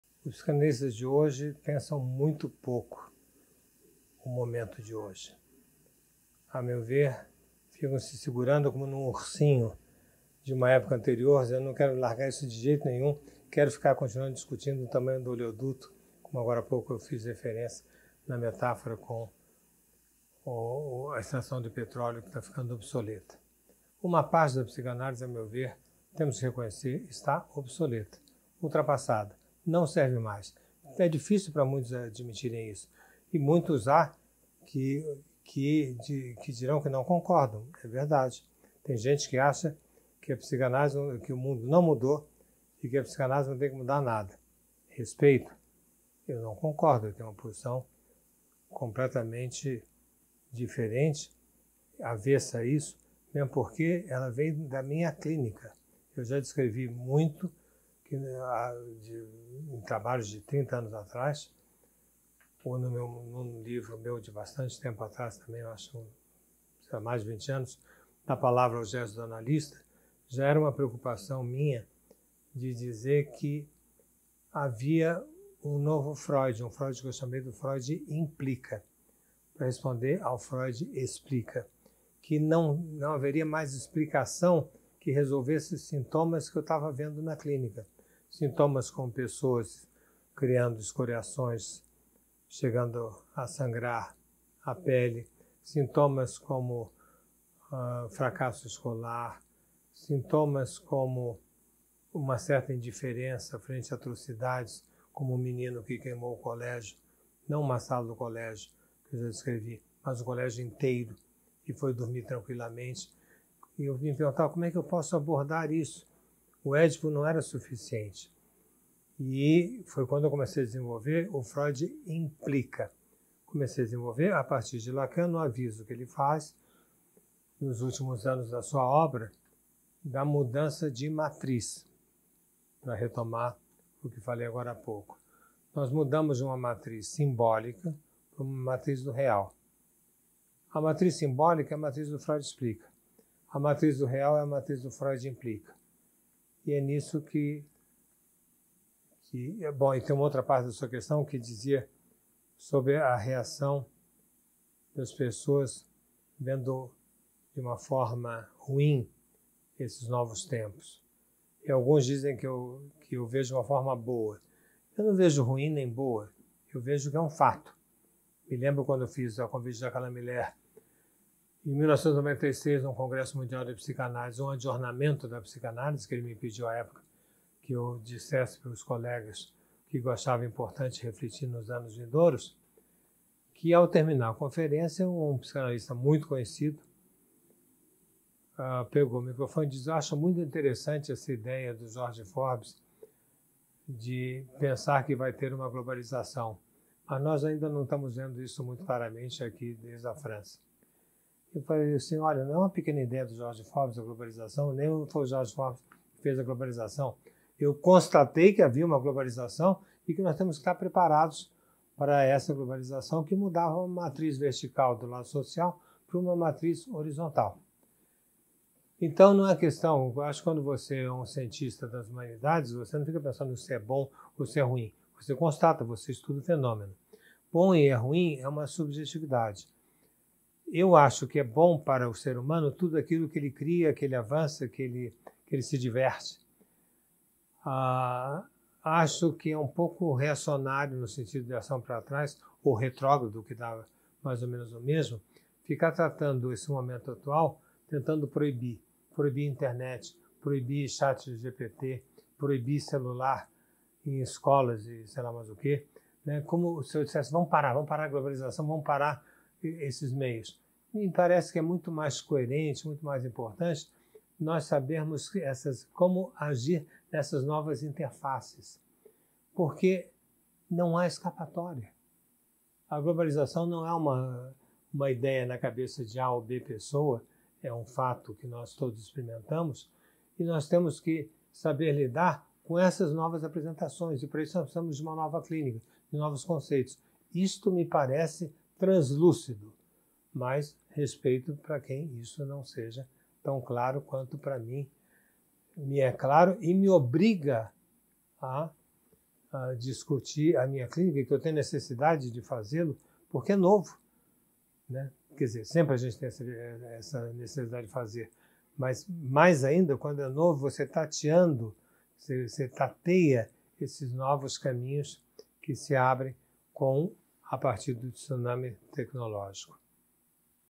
Jorge Forbes: A questões da Psicanálise Hoje
As Identidade, as Fakes News, e a Verdade Mentirosa…
De 06 a 08 de dezembro Jorge Forbes, Eugênio Bucci, Bete Coelho e François Ansermet debatem “Os Desafios em 2025 para a Psicanálise e para os Psicanalistas”.

Quer saber como participar?